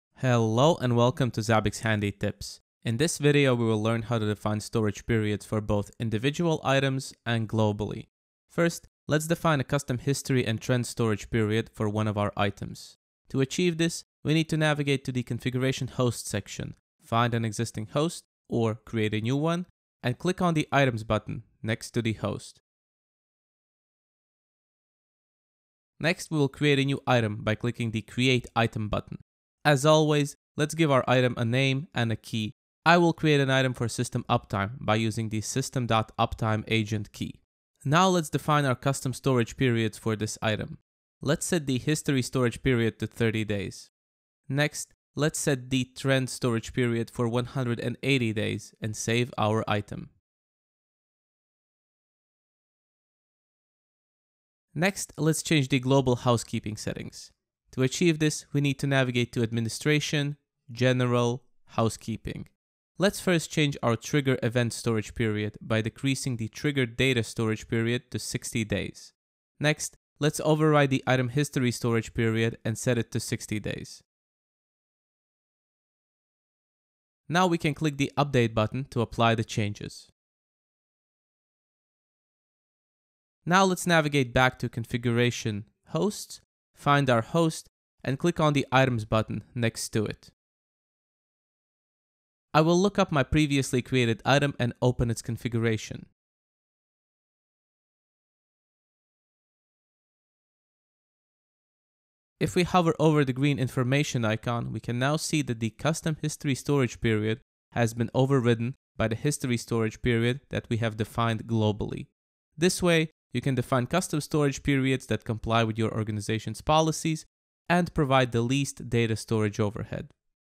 Zabbix Handy Tips: Configuring data retention periods
Zabbix Handy Tips - is byte-sized news for busy techies, focused on one particular topic. In this video, we will take a look at Zabbix item data storage settings and global Housekeeping configuration.

to byte-sized news "Zabbix Handy tips" for busy techies! No spam, ever.